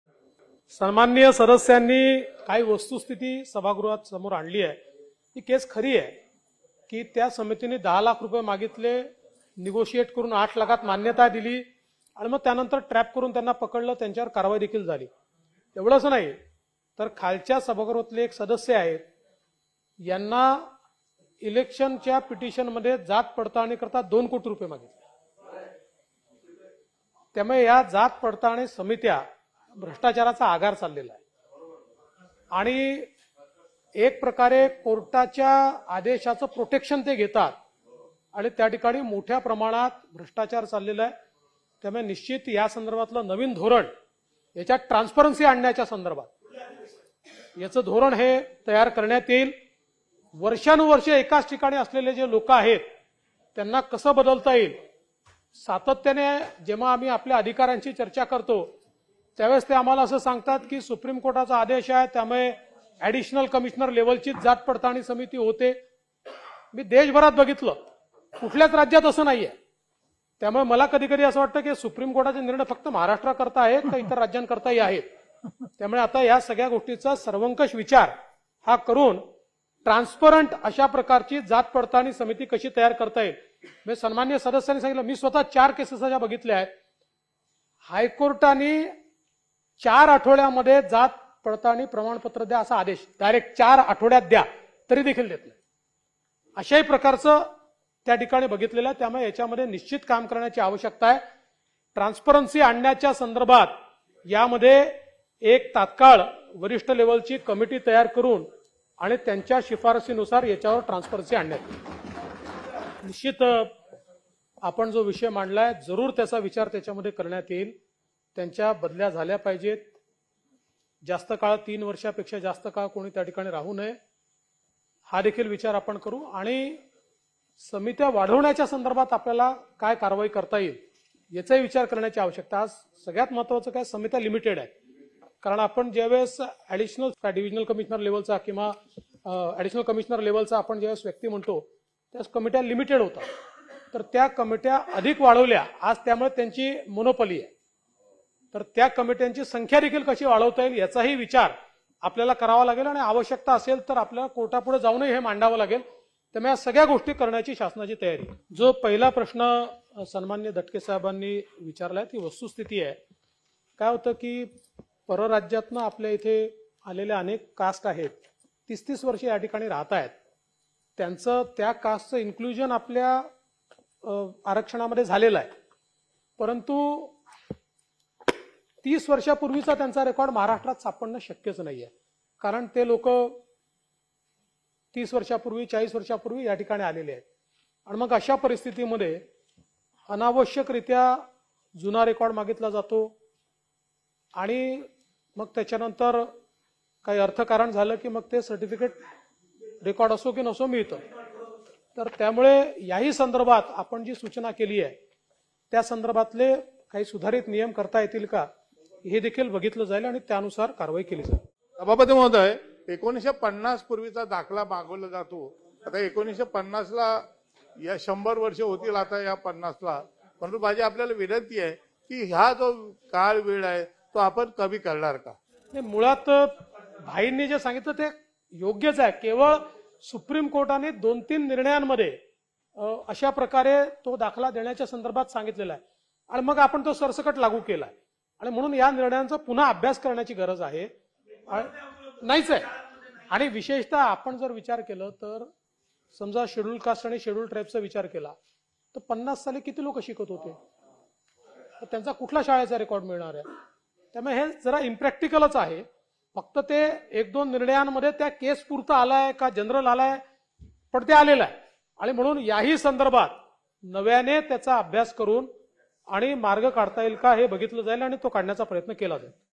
जात पडताळणी समित्यांतील पारदर्शीपणा कारभारासाठी शासन नवे धोरण आणणार | DCM Devendra Fadnavis
जात पडताळणी समित्यांतील पारदर्शीपणा कारभारासाठी शासन नवे धोरण आणणार | DCM Devendra Fadnavis (विधानपरिषद । 3 मार्च 2023)